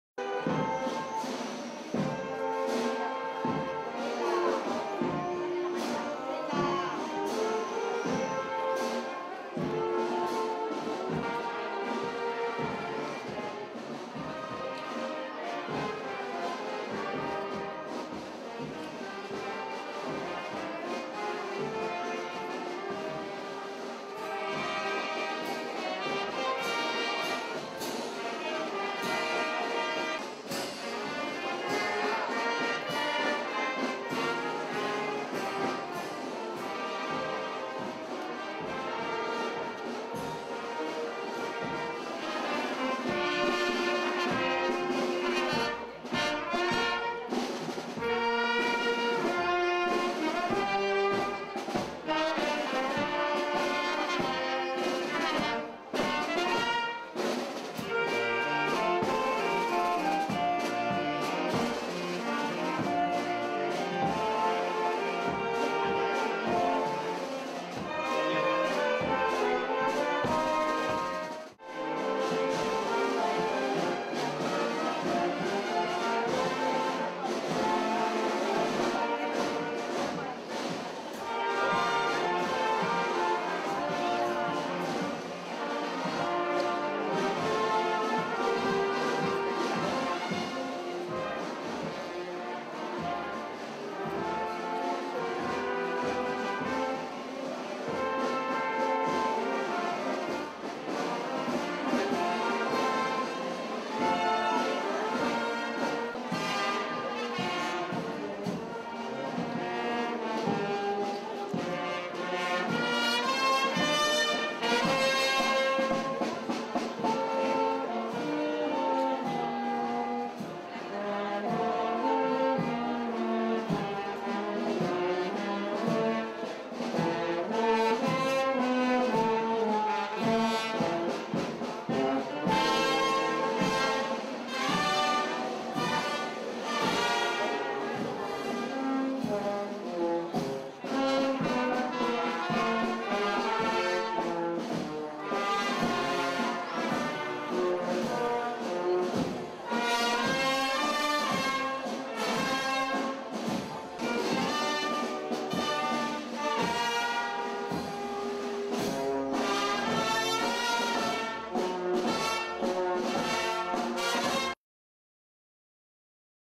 Desfile del Lunes II
Cuadrillas de La Mayor y El Rosel y San Blas en la mañana del Lunes de Bailas